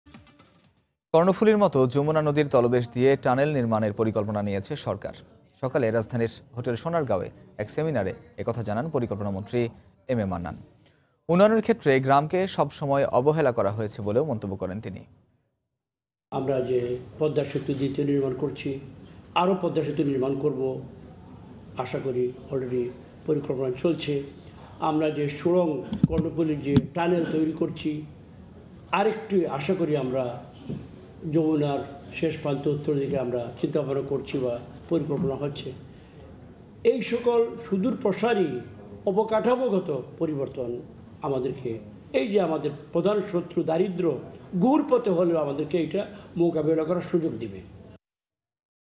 যমুনা নদীর তলদেশ দিয়ে হবে টানেল নির্মাণ | Muhammad Abdul Mannan | Somoy TV
কর্ণফুলির মতো যমুনা নদীর তলদেশ দিয়ে টানেল নির্মাণের পরিকল্পনা নিয়েছে সরকার। সকালে রাজধানীর হোটেল সোনারগাঁওয়ে এক সেমিনারে একথা জানান পরিকল্পনামন্ত্রী এম এ মান্নান। উন্নয়নের ক্ষেত্রে গ্রামকে সবসময় অবহেলা করা হয়েছে বলেও মন্তব্য করেন তিনি।